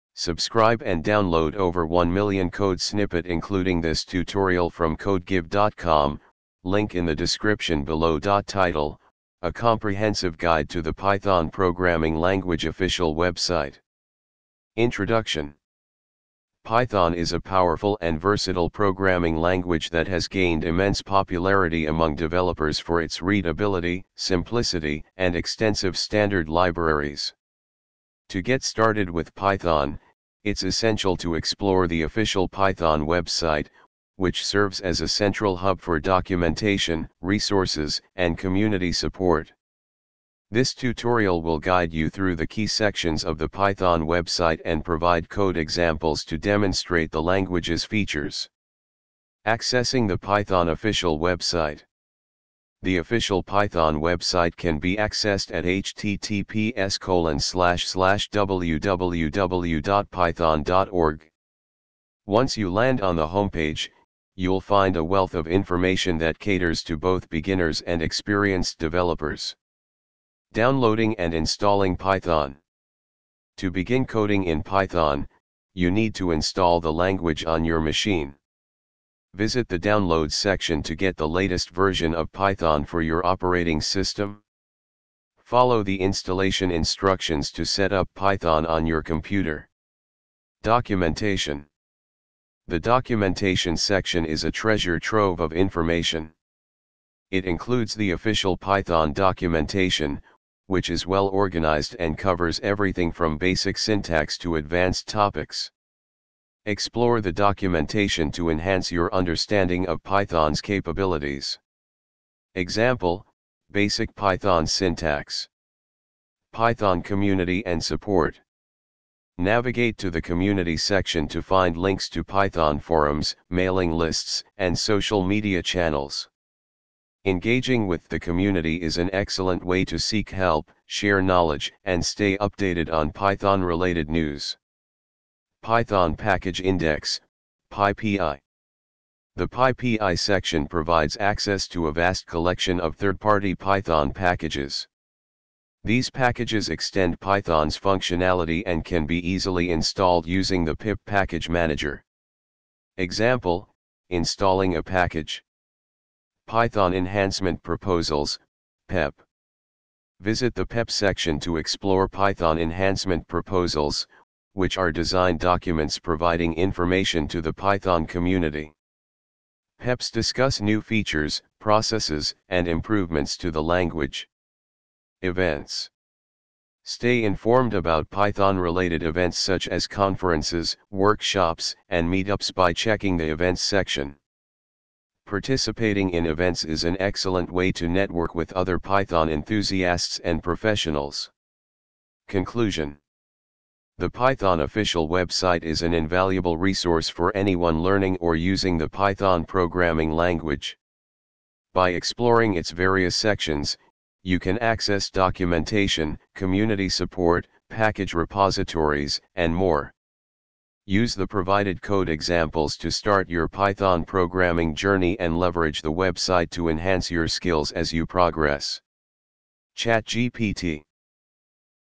python programming language official website
Introduction:
Python is a powerful and versatile programming language that has gained immense popularity among developers for its readability, simplicity, and extensive standard libraries. To get started with Python, it's essential to explore the official Python website, which serves as a central hub for documentation, resources, and community support. This tutorial will guide you through the key sections of the Python website and provide code examples to demonstrate the language's features.
The official Python website can be accessed Once you land on the homepage, you'll find a wealth of information that caters to both beginners and experienced developers.
Downloading and Installing Python:
To begin coding in Python, you need to install the language on your machine. Visit the "Downloads" section to get the latest version of Python for your operating system. Follow the installation instructions to set up Python on your computer.
Documentation:
The "Documentation" section is a treasure trove of information. It includes the official Python documentation, which is well-organized and covers everything from basic syntax to advanced topics. Explore the documentation to enhance your understanding of Python's capabilities.
Example: Basic Python Syntax
Python Community and Support:
Navigate to the "Community" section to find links to Python forums, mailing lists, and social media channels. Engaging with the community is an excellent way to seek help, share knowledge, and stay updated on Python-related news.
Python Package Index (PyPI):
The "PyPI" section provides access to a vast collection of third-party Python packages. These packages extend Python's functionality and can be easily installed using the pip package manager.
Example: Installing a Package
Python Enhancement Proposals (PEP):
Visit the "PEP" section to explore Python Enhancement Proposals, which are design documents providing information to the Python community. PEPs discuss new features, processes, and improvements to the language.
Events:
Stay informed about Python-related events such as conferences, workshops, and meetups by checking the "Events" section. Participating in events is an excellent way to network with other Python enthusiasts and professionals.
Conclusion:
The Python official website is an invaluable resource for anyone learning or using the